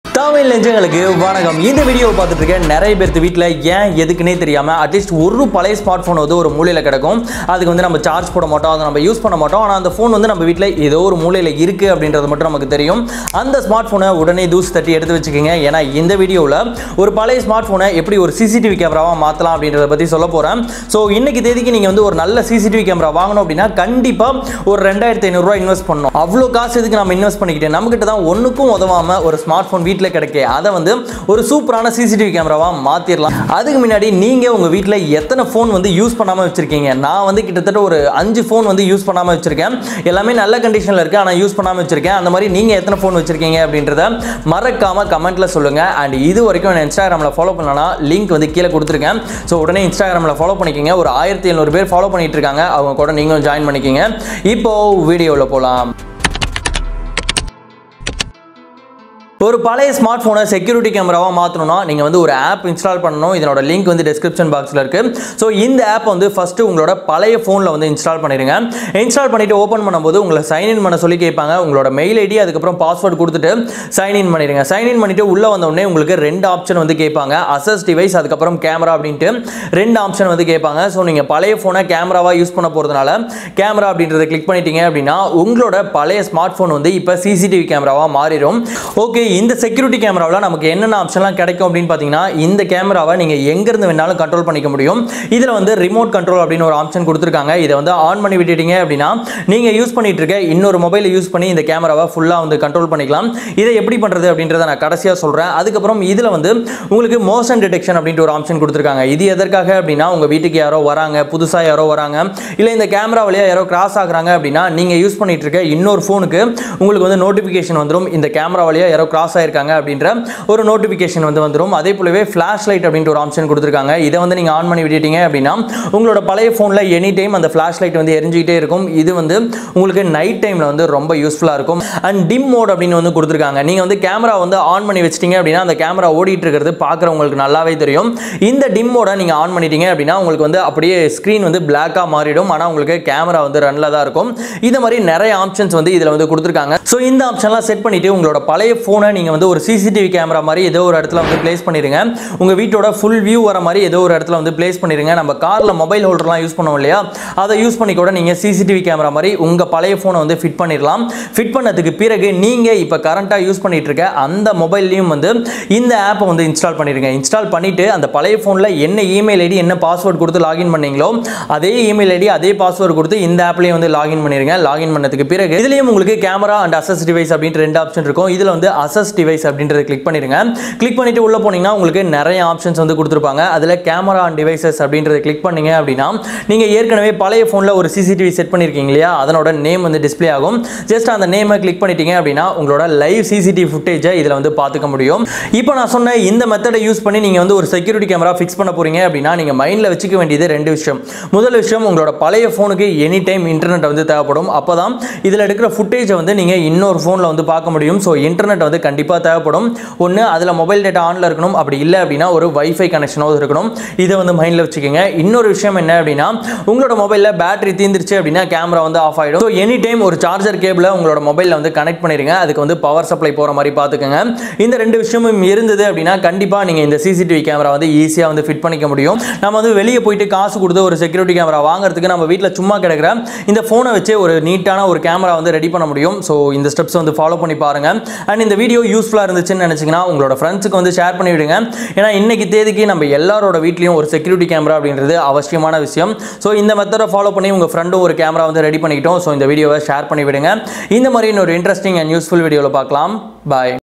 CCTV Camera On Android Mobile | Convert Old Mobile As Security Camera
Le's Be Friends On